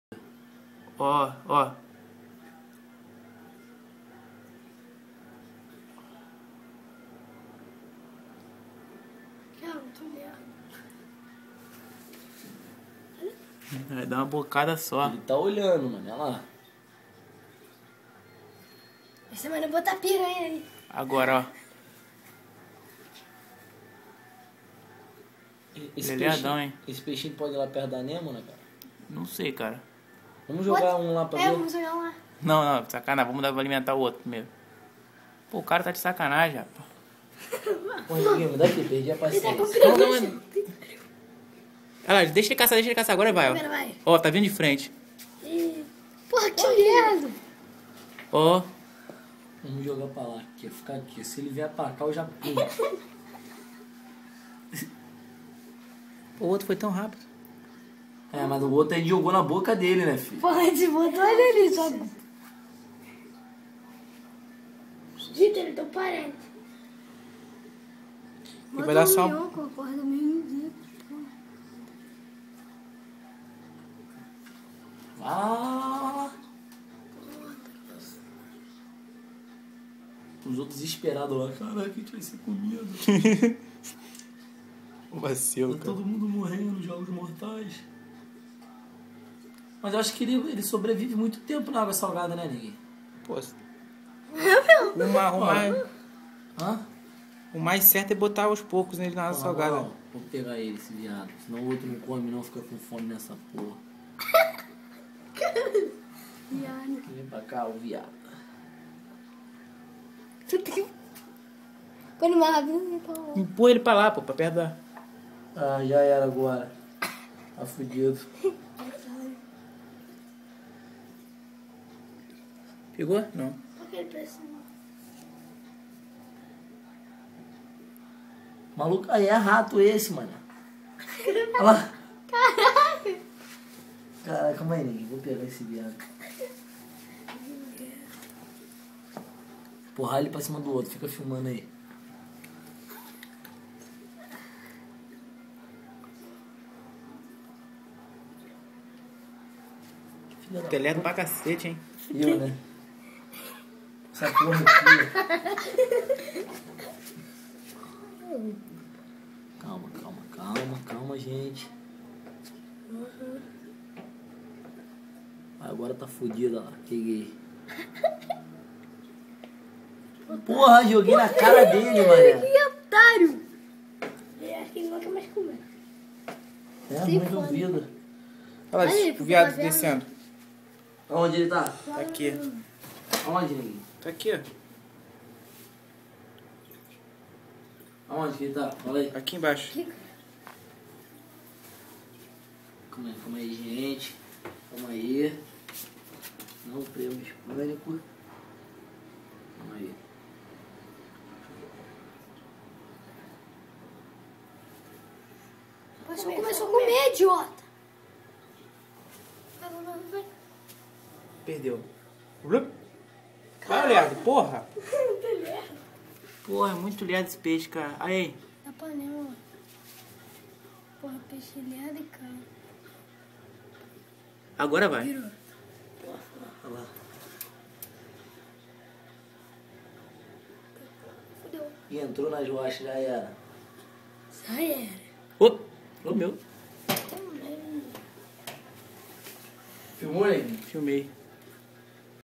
Lionfish se alimentando
Dando comida para o meu Lionfish.
Foi a primeira vez que ele comeu em meu aquario, ainda é filhote por isso a lerdeza dele.

DEpois de muita dificuldade, tive que colocar o peixe quase que na boca dele. Agora peguei o macete !

Fotos : flickr vivi_seaworld